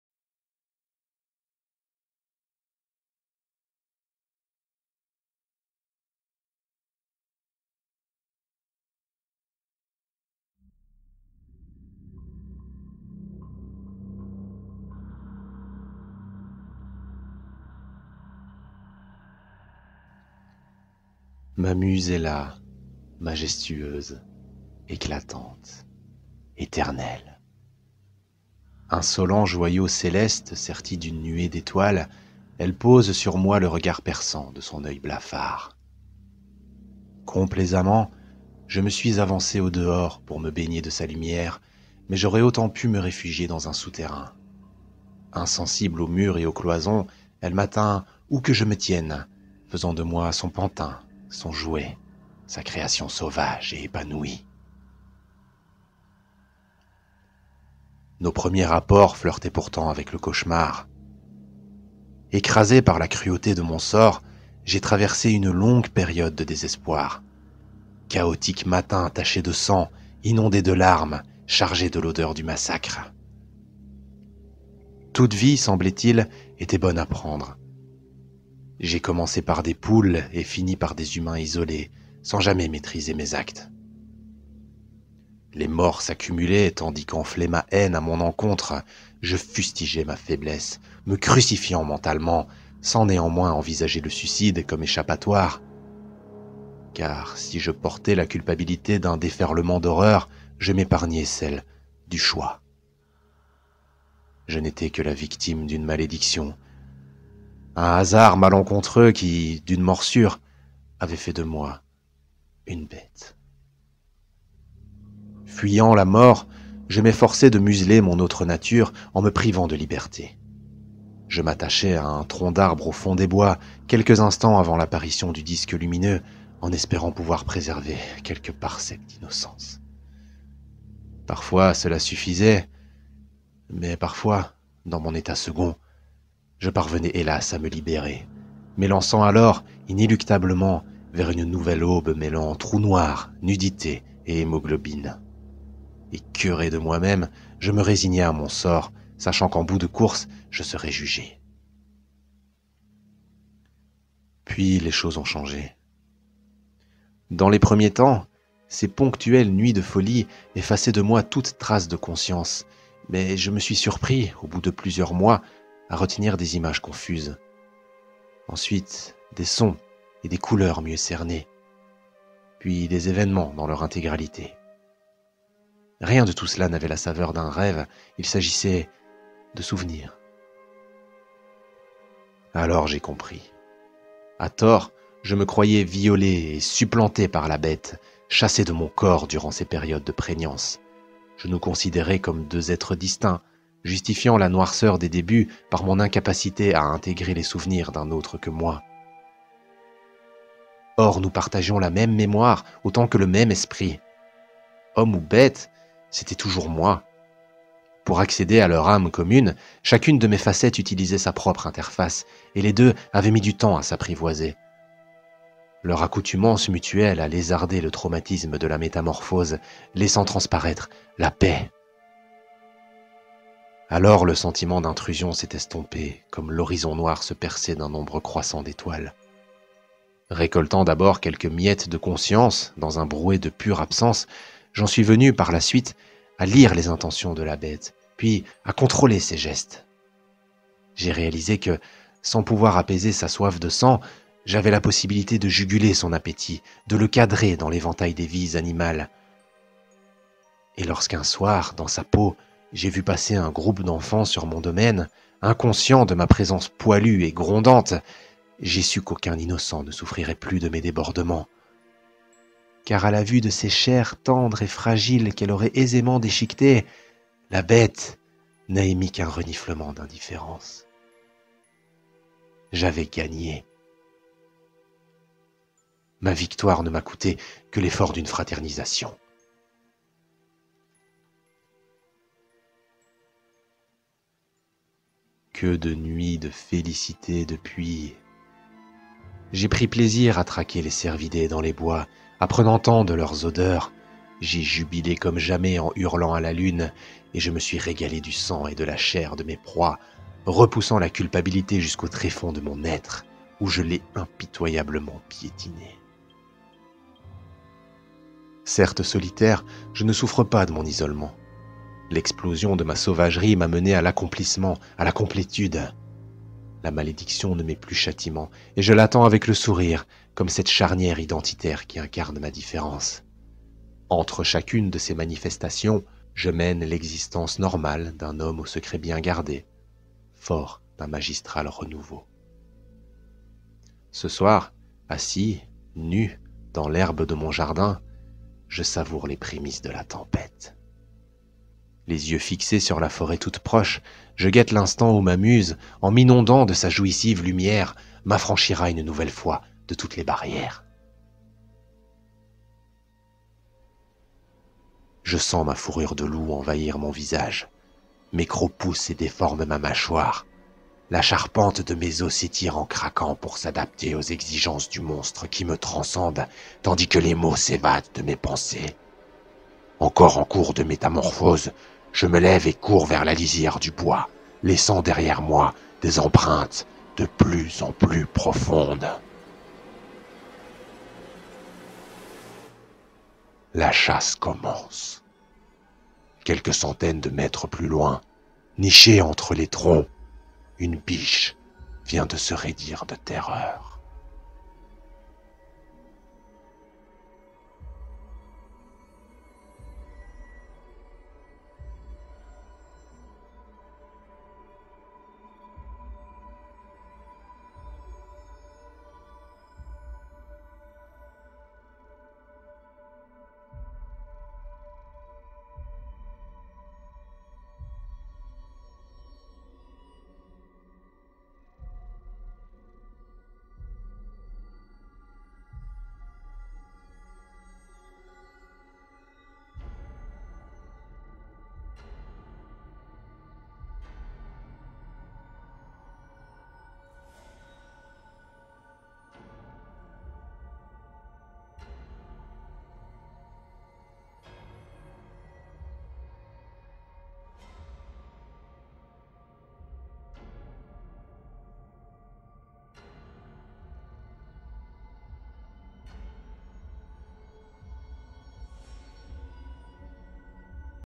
MA MUSE EST BESTIALE récit audio de loup-garou
Une histoire audio dans le genre fantastique et horreur. Quand une malédiction réveille la bête, il n'y a rien d'autre à faire qu'à s'y accoutumer...

Liste des musiques utilisées